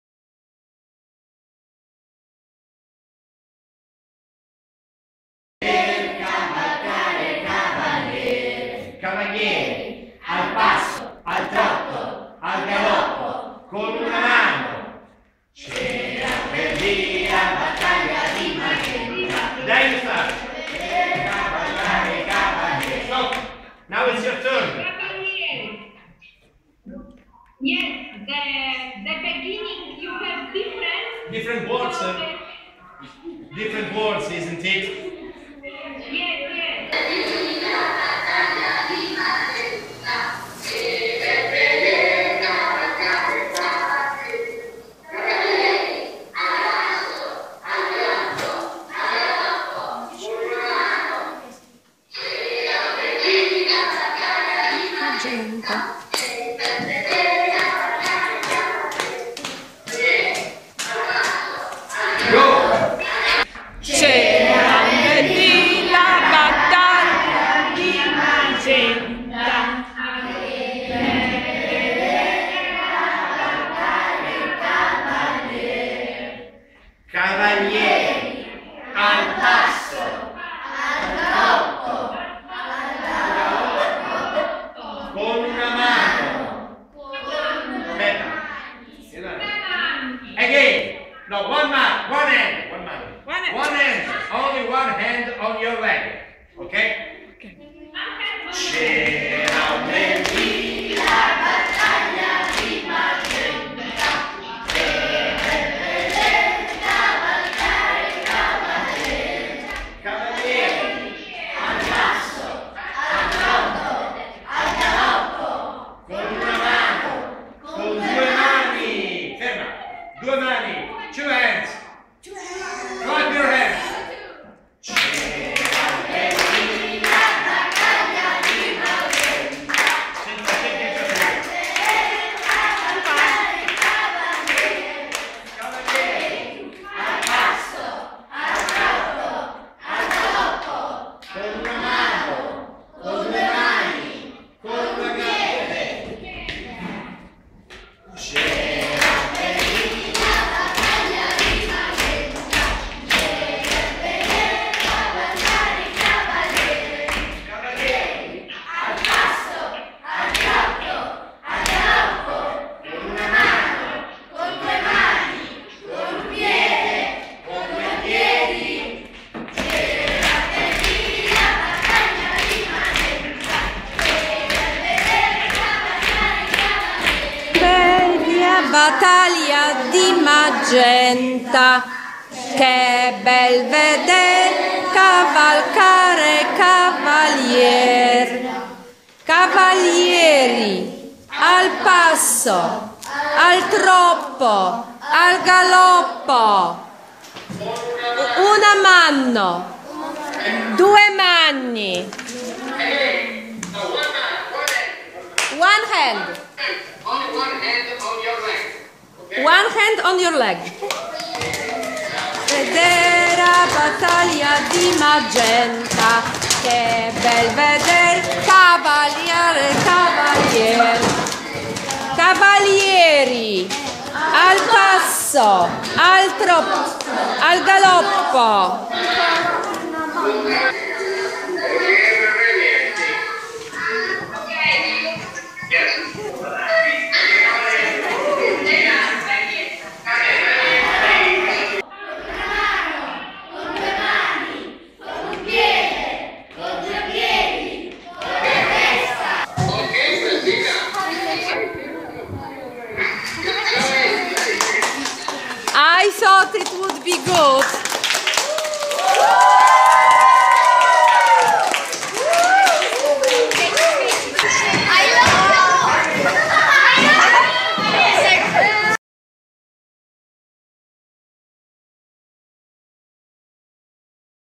Videoconference Italy - Poland Cavalieri di Magenta
LLP Comenius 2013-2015 Project Children's Eyes, Children's Hearts
Videoconference of the Italian and Polish schools to celebrate Italian Week